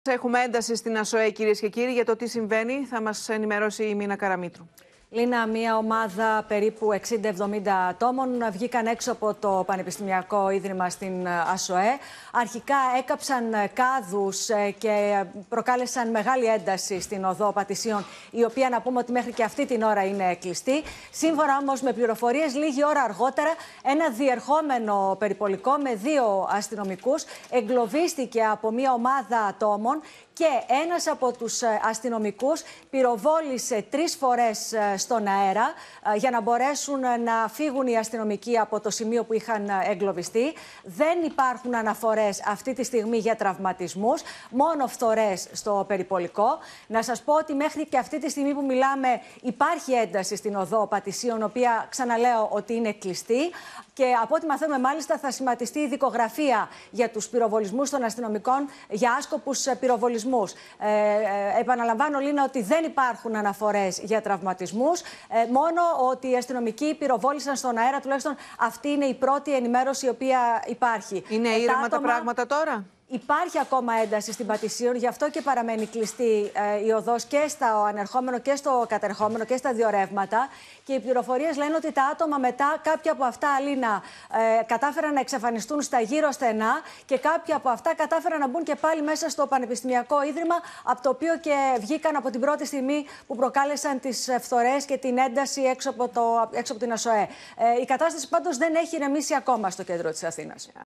Επεισόδια έξω από την ΑΣΟΕΕ:Εγκλωβίστηκαν αστυνομικοί, πληροφορίες για πυροβολισμό στον αέρα|OPEN TV
Γίνετε μέλος στο κανάλι του OPEN για να αποκτήσετε πρόσβαση σε προνόμια:
Έβαλαν φωτιές σε κάδους έξω από την ΑΣΟΕΕ.